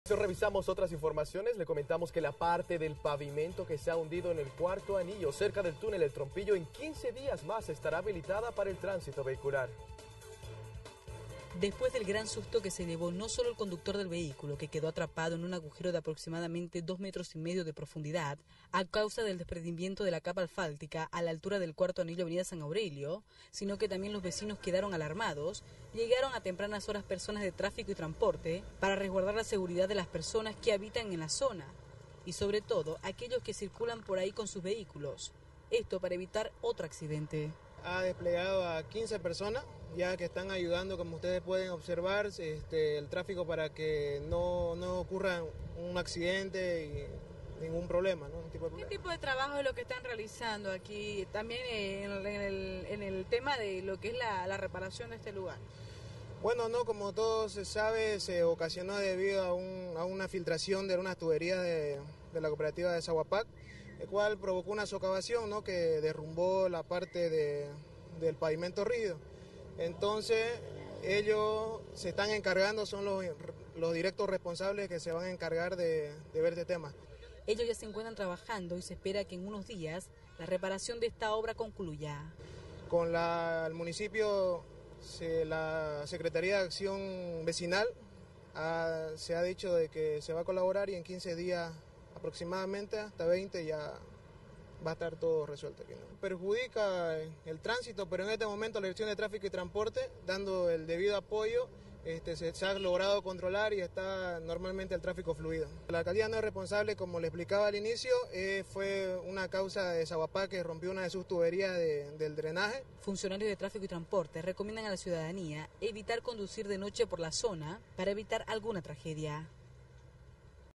Reparación de pavimento hundido demorará 15 días
La parte del pavimento que se hundió en el Cuarto Anillo cerca del Túnel Trompillo demorará 15 días en estar habilitada nuevamente para el tránsito vehicular. El agujero alcanza los dos metros de profundidad. Funcionarios de Tráfico y Transporte recomendaron no circular de noche por la vía